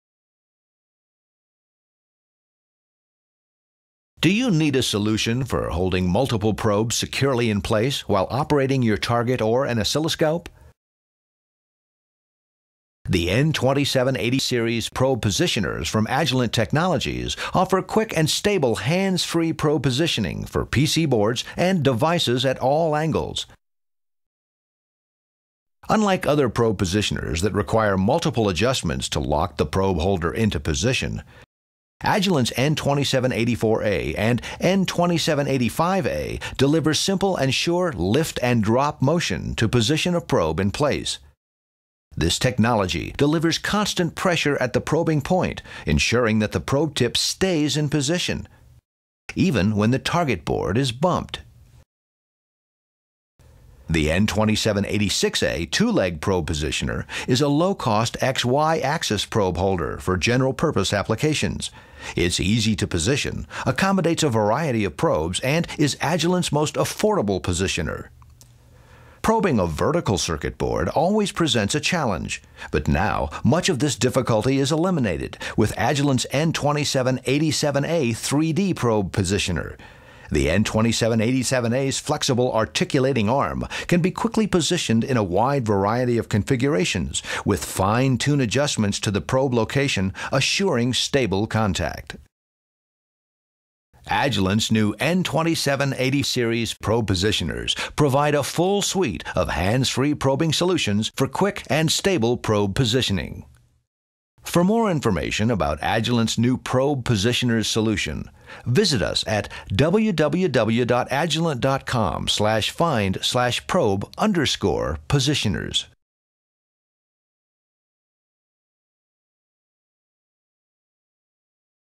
Agilent Probe Positioners Video Demo
The Agilent's N2784A-N2787A Series probe positioners provide quick and stable probe positioning to PC boards and devices that require hands-free probing.
Unlike other probe positioners that require multiple adjustments to lock the probe holder into position, the N2784A and N2785A need only a lift and drop motion to position the probe in place. The weight stabilization technique used in the probe holder keeps constant pressure at the probing point so the probe tip stays in position even when the target board is bumped.
The N2786A is a low-cost, easy-to-use XY axis probe holder for general purpose probing applications. The two-legged positioner is designed to be easy-to-use  the positioner itself has no controls to position it in place.
The N2787A is a 3D probe positioner with a flexible, articulating arm that can be quickly positioned in a wide variety of configurations. At the same time it also allows fine-adjustment to the probe location to obtain stable contact.
These probe positioners fit most Agilent passive and active probes and are also compatible with various probes manufactured by other vendors.